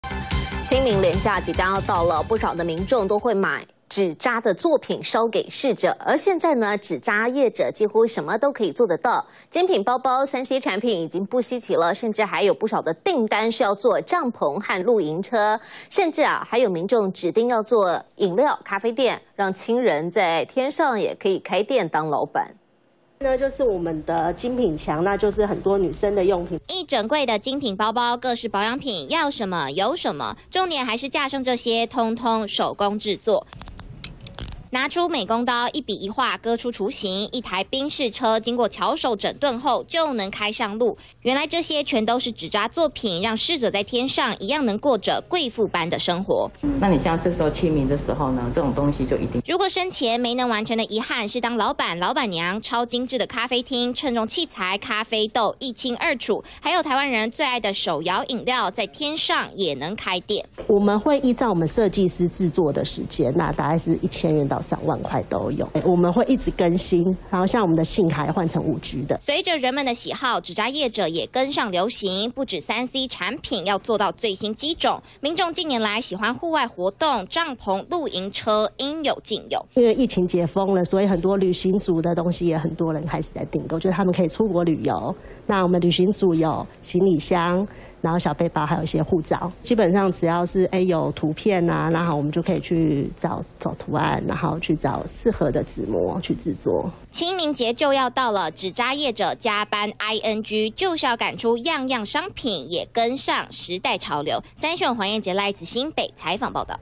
燒一間咖啡廳給你！手搖飲料店也能燒？清明掃墓祭祖不只能燒車子、名牌包給親友！想要Switch、帳篷、露營車都能幫你做｜消費新聞｜三立iNEWS李昕芸 主播｜投資理財、財經新聞 都在94要賺錢
清明節要到了，不少民眾會買紙紮作品燒給逝者，現在紙紮業者幾乎什麼都做得到，精品包包、3C產品已經不稀奇，還有不少訂單是要做帳篷和露營車，也有民眾特別指定要做手搖飲料、咖啡店，讓親人在天上也能開店當老闆。